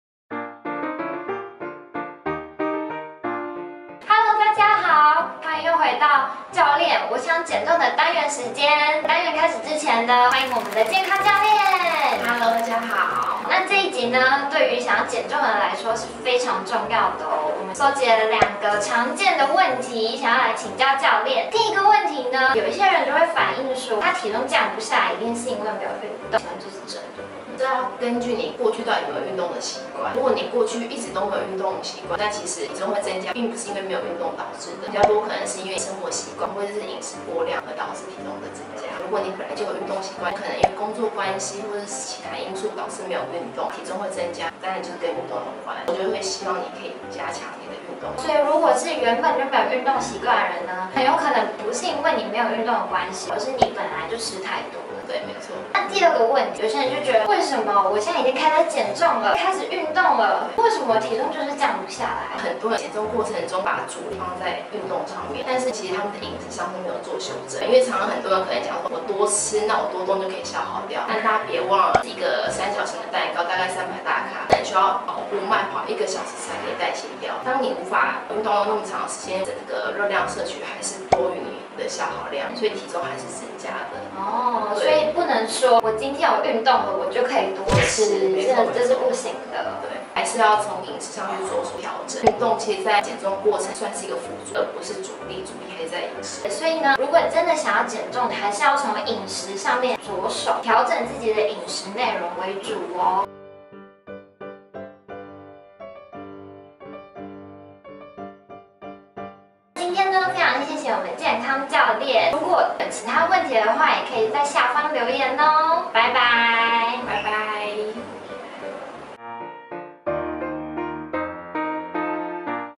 減重的兩大常見問題【 教練！我想減重 EP3 】
許多人在減重時都會遇到瓶頸，除了保持運動的習慣外，還需要注意什麼呢？
第三集【 教練！我想減重 】的單元，為大家整理了減重時常見的問題。
瘦不下來一定是因為我沒有運動嗎？只要我有運動，吃什麼都可以嗎？多動就可以多吃嗎？
快和我們一起破除減重的迷思，擁有正確觀念並且有效的減重。